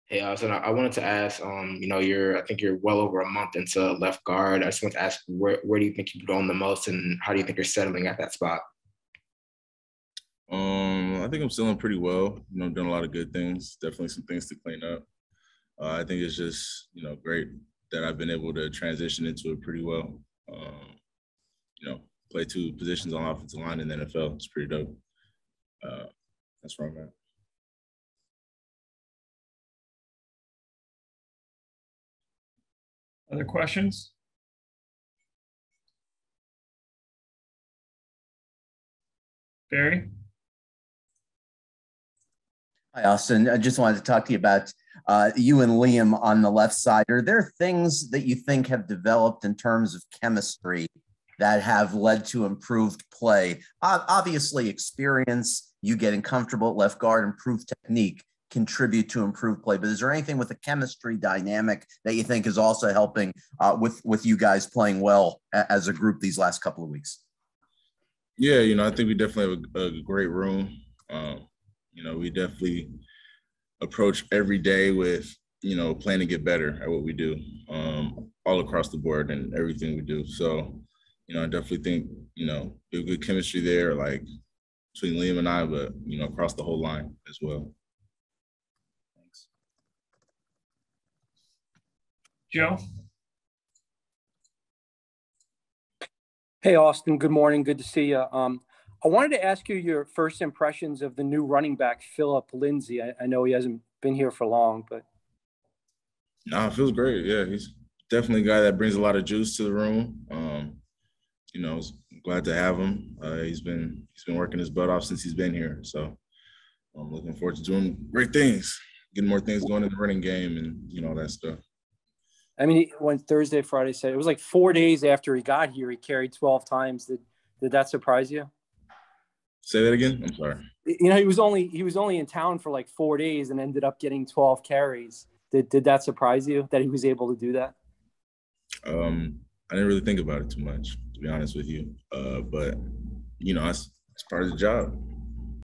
Austin Jackson meets with the media | Miami Dolphins
Hear from Austin Jackson as the team prepares for the Giants.